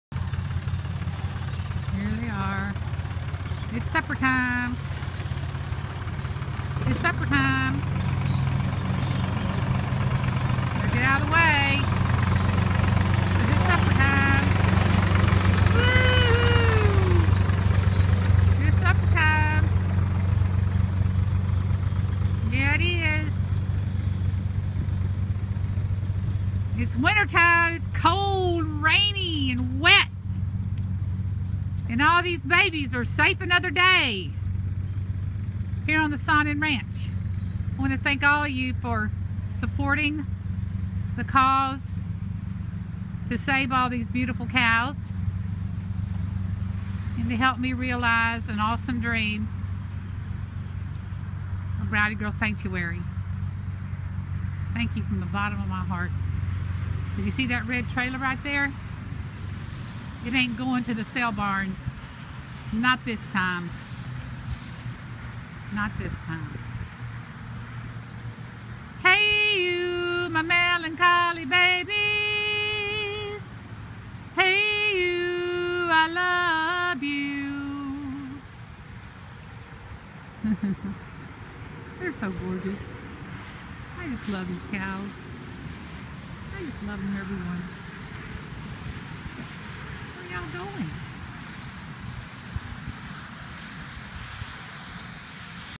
I Just Love These Cows
Supper time. Cold, wet and rainy !! They're safe another day. Can't do it without you.